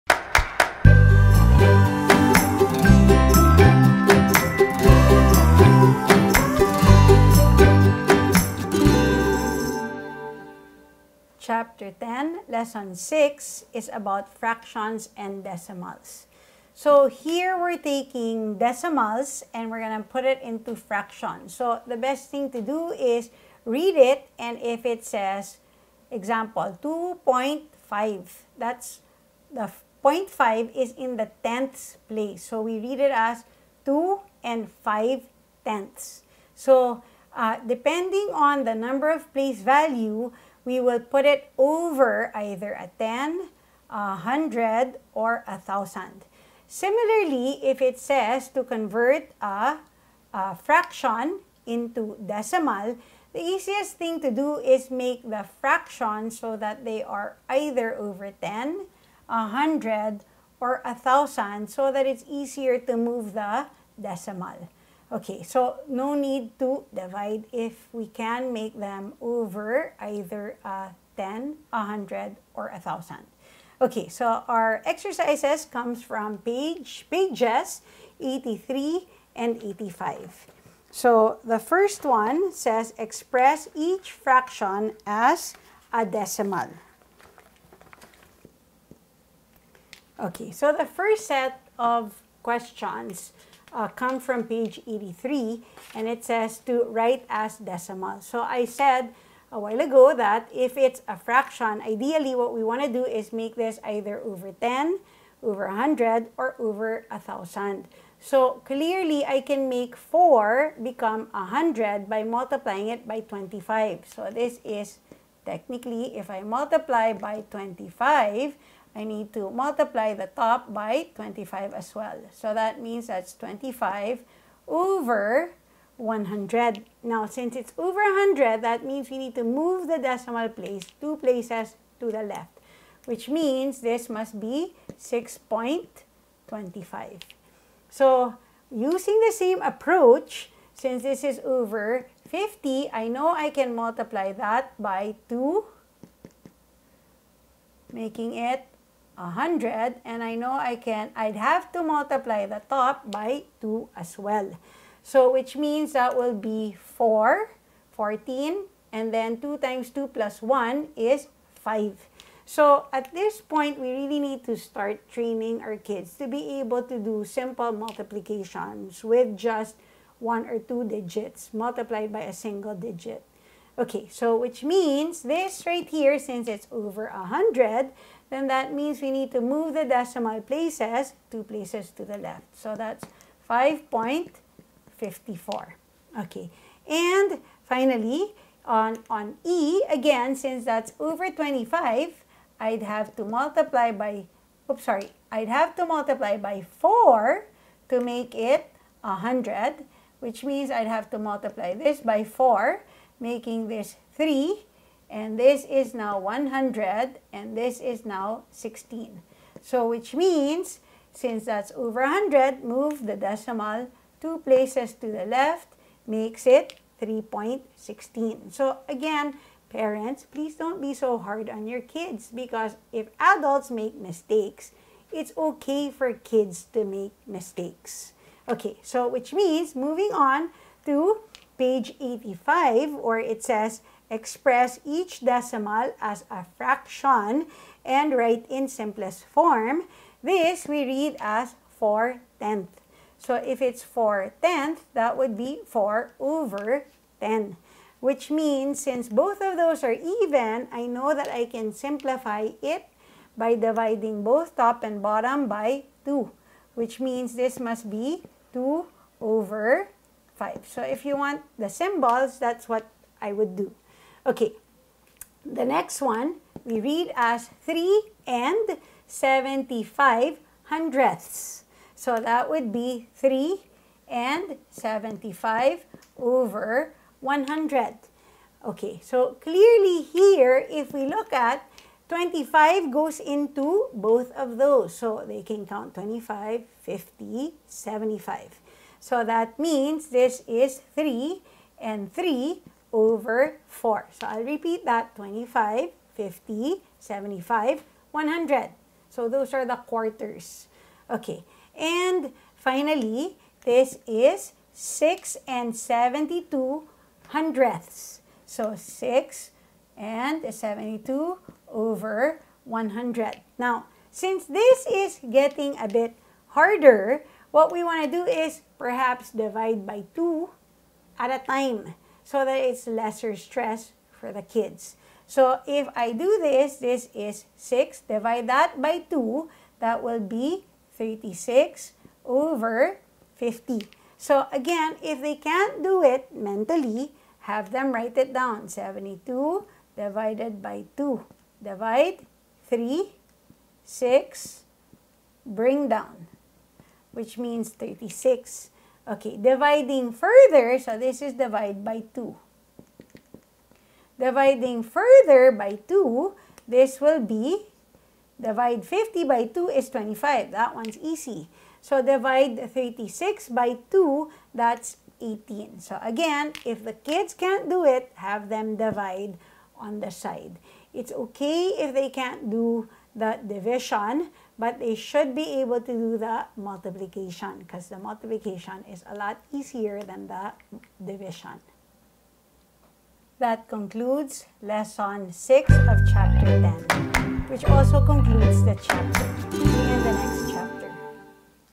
Singapore Math: Grade 4 - Fractions and Decimals
Instructional video on Singapore Math Grade 4 by Michelle Reyes based on the curriculum of Marshall Cavendish Education's "My Pals are here! - Maths 4B" book.

Chapter 10: Decimals
Lesson 1.6: Fractions and Decimals